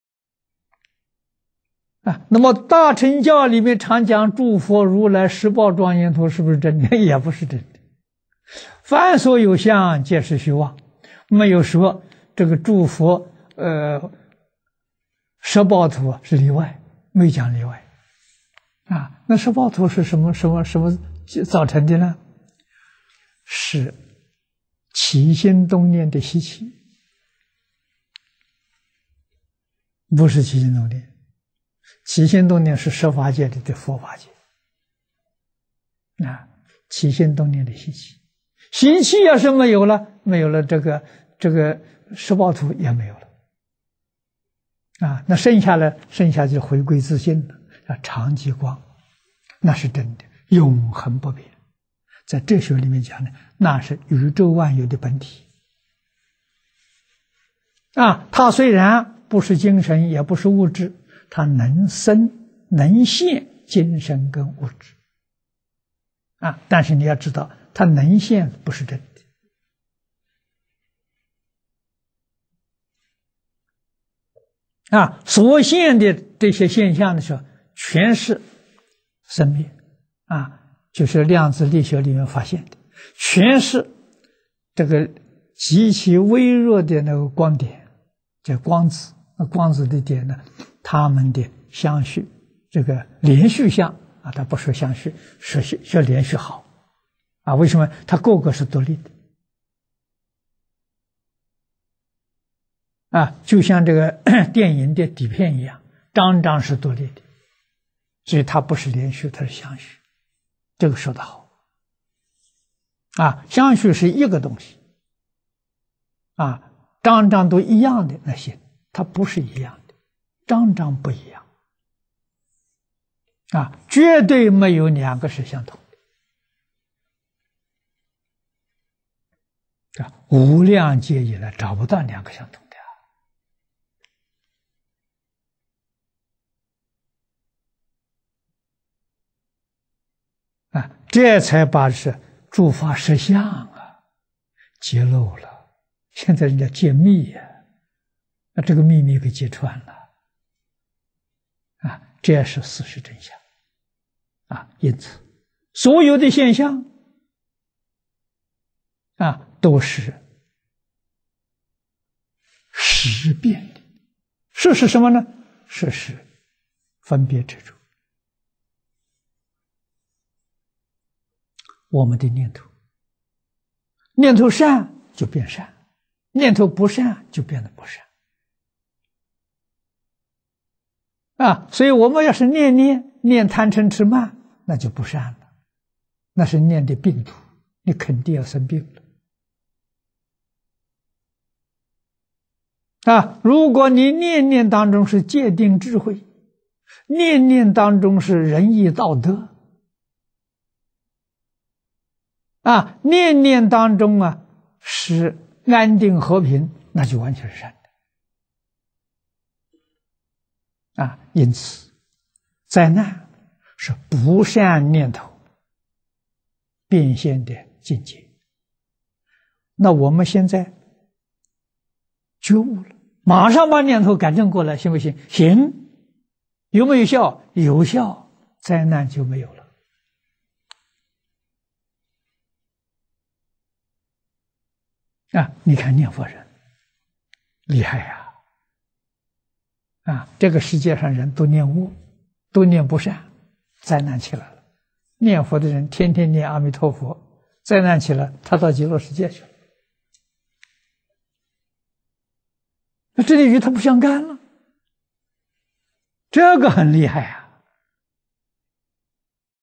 念佛人的真功夫
大乘教裡面常講諸佛如來實報莊嚴土是不是真的？也不是真的。「凡所有相，皆是虛妄」，沒有說諸佛實報土是例外，沒有講例外。實報土是什麼造成的？是起心動念的習氣，不是起心動念。起心動念是十法界裡的佛法界，起心動念的習氣；習氣要是沒有了，這個實報土也沒有了，剩下來的就回歸自性，叫常寂光，那是真的，永恆不變，在哲學裡面講那是宇宙萬有的本體，它雖然不是精神也不是物質，它能生能現精神跟物質。但是你要知道，它能現不是真的，所現的這些現象全是生滅，就是量子力學裡面發現的，全是極其微弱的光點，光子，光子的點它們的連續相。它不說相續，說連續好，為什麼？它各個是獨立的。就像電影的底片一樣，張張是獨立的，所以它不是相續，它是連續的，這說得好。相續是一個東西，張張都一樣的，那行；它不是一樣的，張張不一樣，絕對沒有兩個是相同的，無量劫以來找不到兩個相同的。這才把諸法實相揭露，現在人講揭祕，這個祕密被揭穿了，這是事實真相。因此，所有的現象都是識變的，識是什麼？識是分別執著。我們的念頭，念頭善就變善，念頭不善就變得不善。所以我們要是念念念貪瞋痴慢，那就不善，那是念的病毒，你肯定要生病。如果你念念當中是戒定智慧，念念當中是仁義道德，念念當中是安定和平，那就完全是善的。
　　因此，災難是不善念頭變現的境界。我們現在覺悟了，馬上把念頭改正過來行不行？行，有沒有效？有效，災難就沒有了。你看念佛人厲害，這個世界上的人都念惡，都念不善，災難起來了；念佛的人天天念阿彌陀佛，災難起來，他到極樂世界去了，這裡與他不相干，這個很厲害！